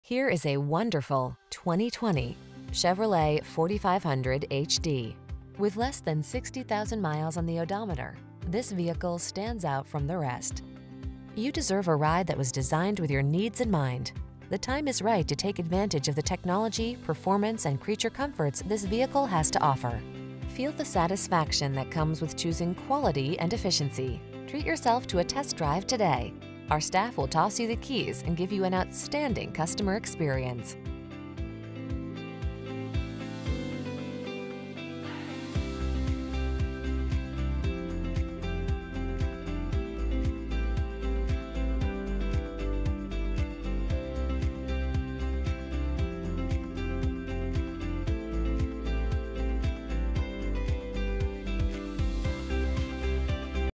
2020 Chevrolet Silverado_4500HD Work Truck GA Gainesville, Atlanta, Cummings, Athens, Dawsonvil...
Pre-Owned 2020 Chevrolet Silverado_4500HD Work Truck available at John Megel Chevrolet located in 1392 Hwy 400 South, Dawsonville, GA, 30534. Servicing the Gainesville, Atlanta, Cummings, Athens, Dawsonville area.